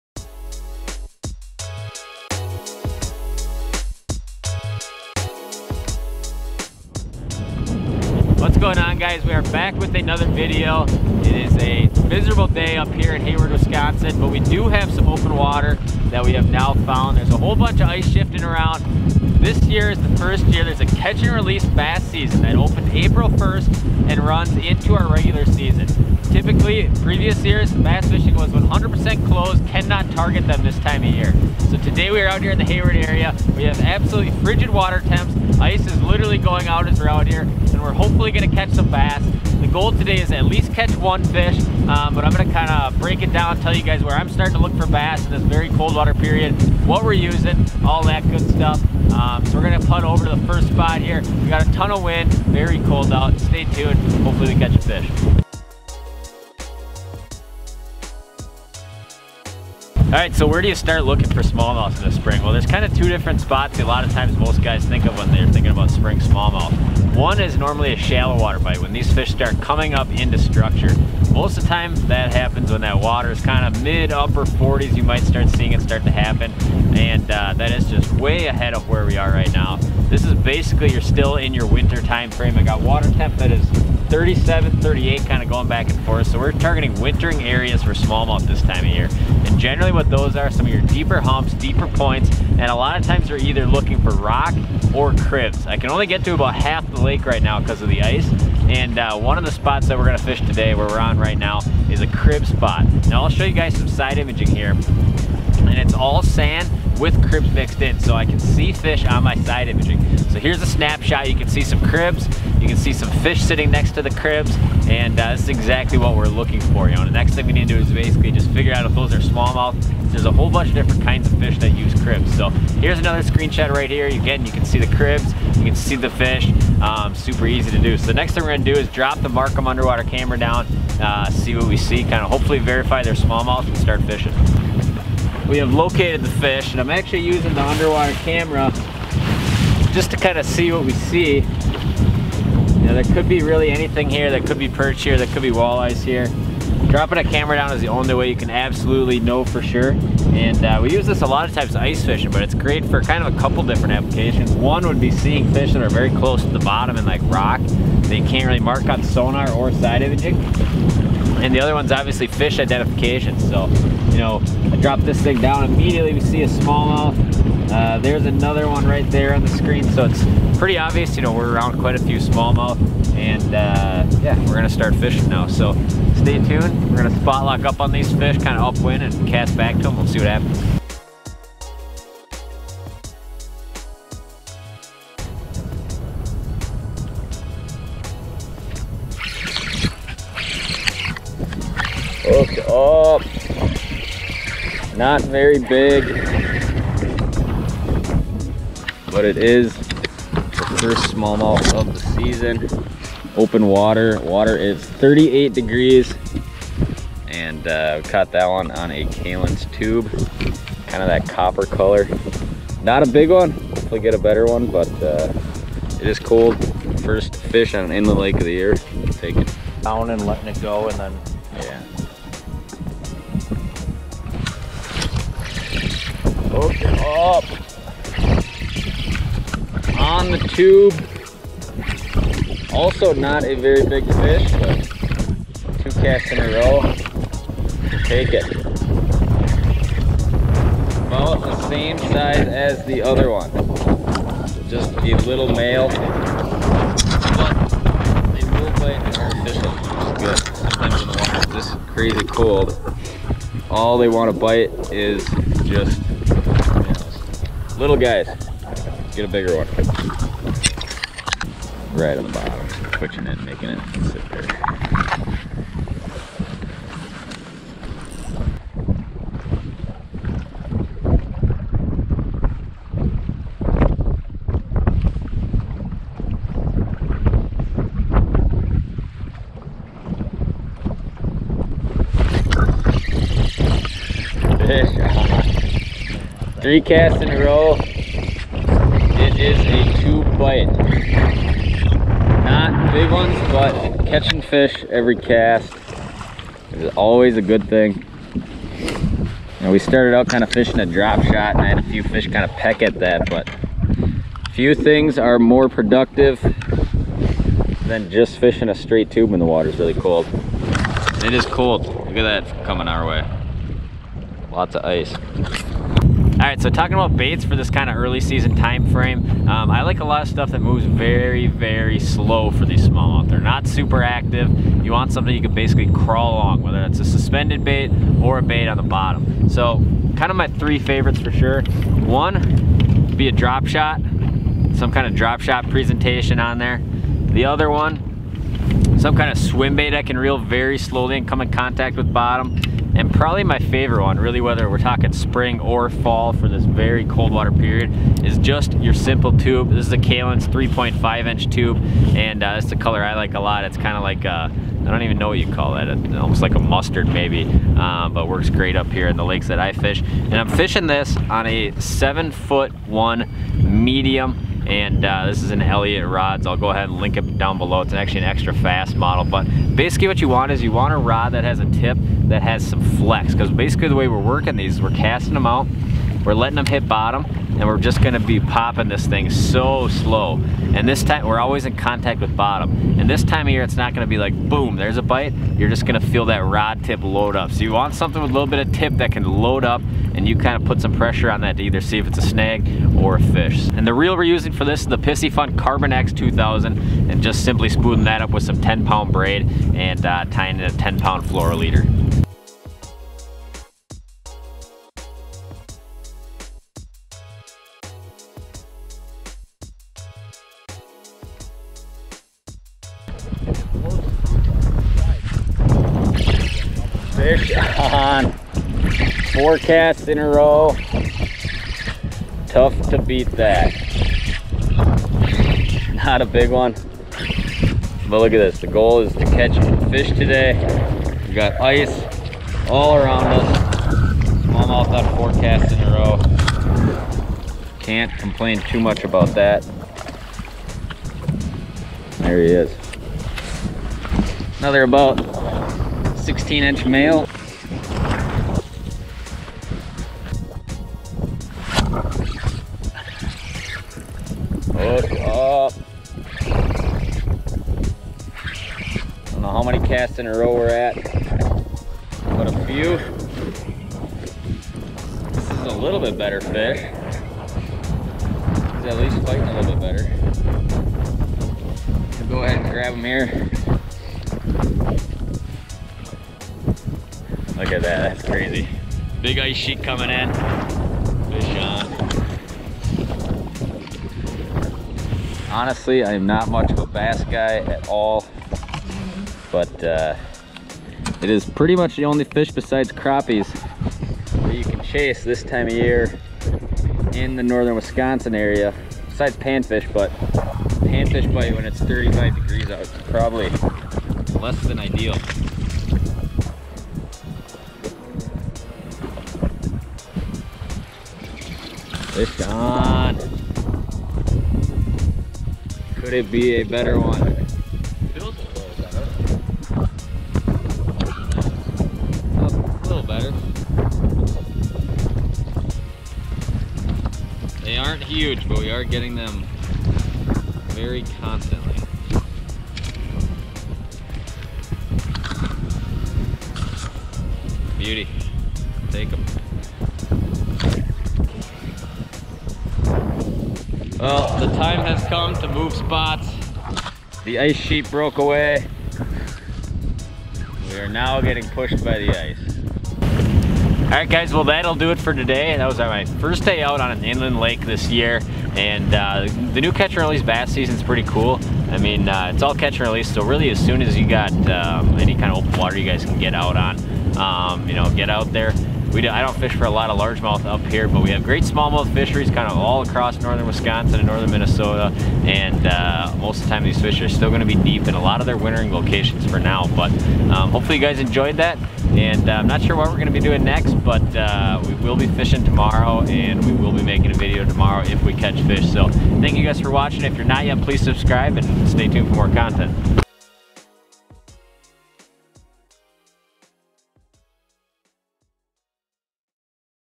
Early Spring Bass Fishing Tips
Today we are bass fishing in northern Wisconsin. This is the first year we have a catch and release bass fishing season the runs throughout the month of April. This is a great opportunity to get out and tarted smallmouth in many of their wintering locations. In this video I talk about some of my favorite spring bass lures as well as how to find and catch smallmouth bass when the water is freezing cold.